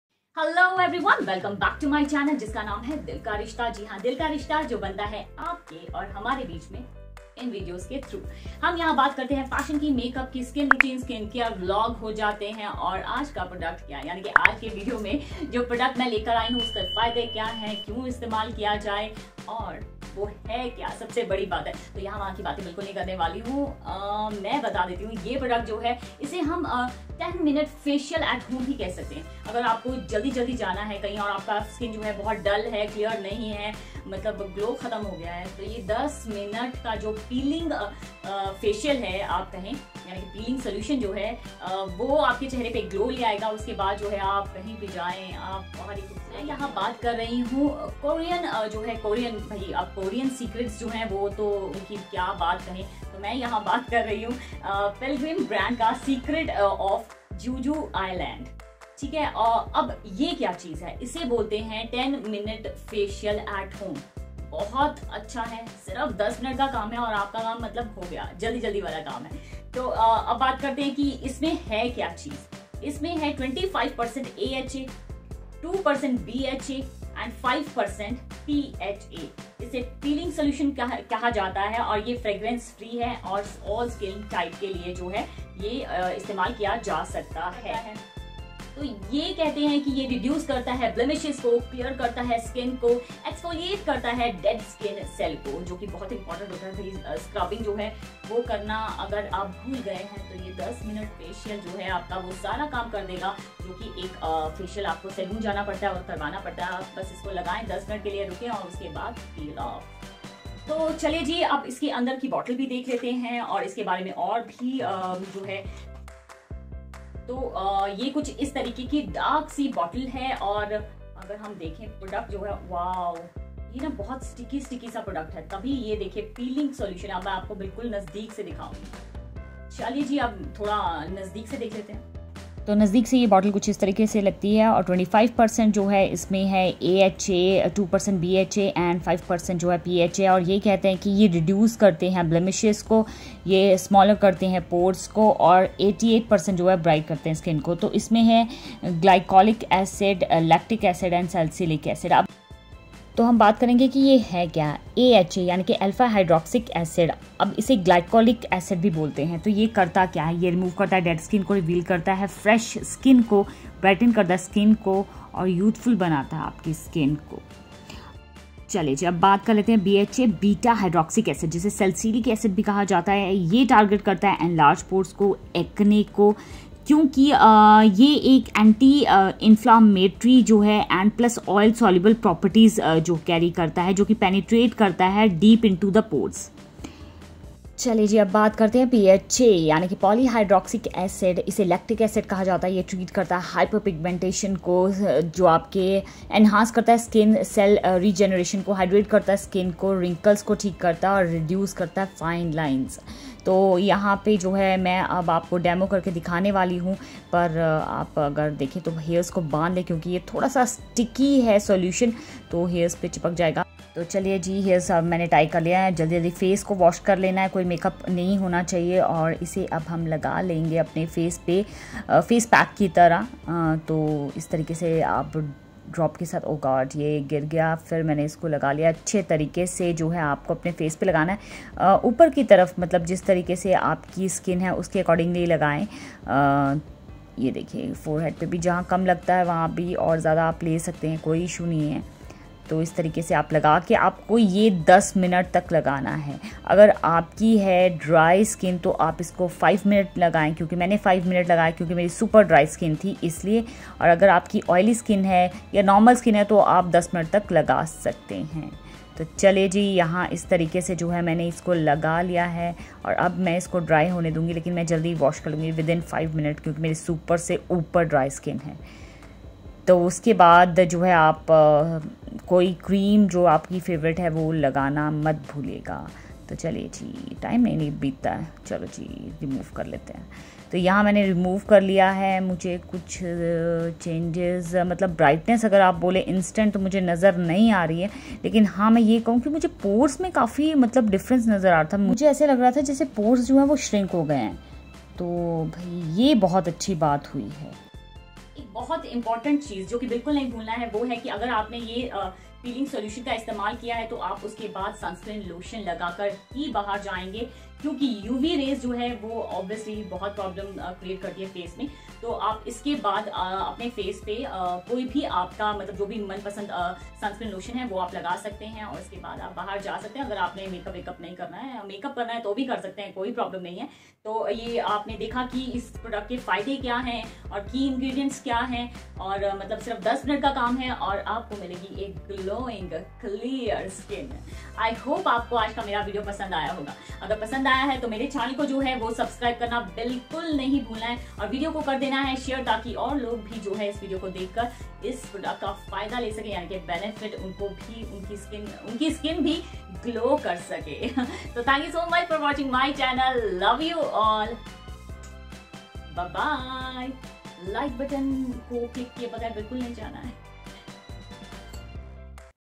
Pilgrim Peeling Solution Review 2022 | Pilgrim AHA BHA PHA Peeling Solution#koreansecret
10 Mins Peeling Facial Peel Pilgrim AHA - BHA - PHA Peeling Solution